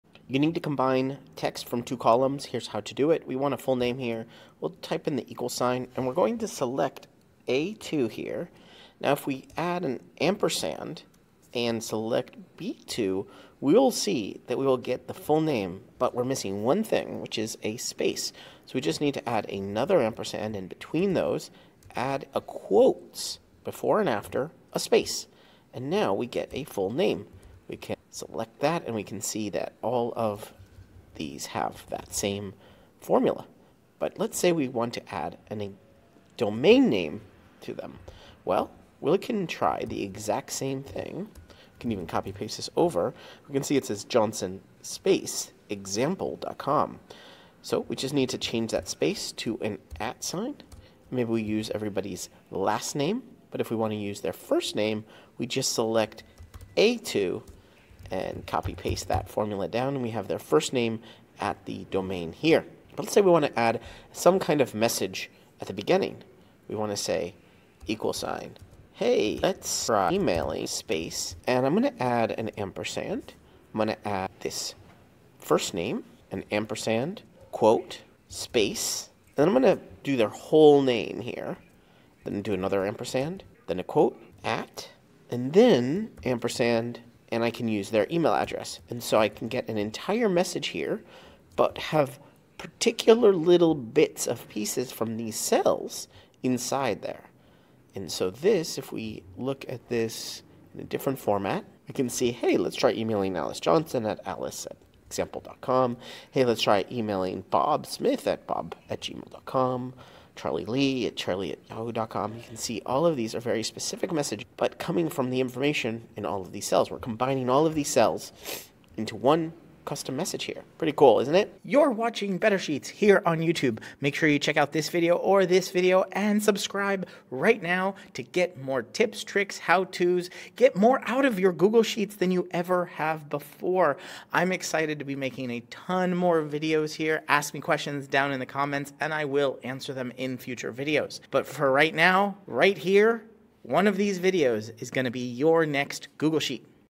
How To Combine Two Columns of Data in Google Sheets
In this video, learn how to combine text from two columns in Google Sheets to create full names and custom messages. We'll go through step-by-step instructions to concatenate cells using formulas, add spaces, and append domain names. By the end, you'll know how to generate personalized messages using data from your sheets. Watch now for detailed insights and become more efficient with your Google Sheets.

00:00 Combining Text from Two Columns
00:27 Adding a Domain Name
01:02 Creating Custom Messages
02:01 Conclusion and Next Steps